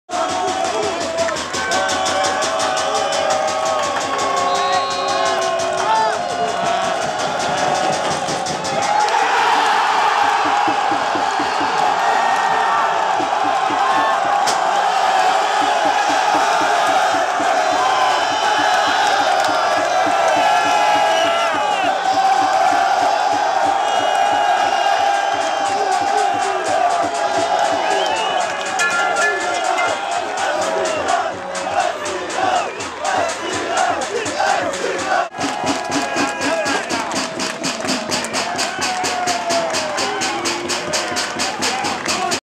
AC vs KC Elvir Kafedzic Goal!
Elvir Kafedzic scores a goal for AC St. Louis!